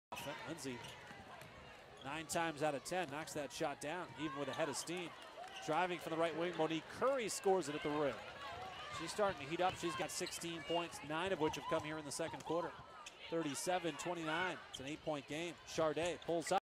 Monique Currie Goes To the Rack
Monique Currie dribbles behind-the-back to get to the hole.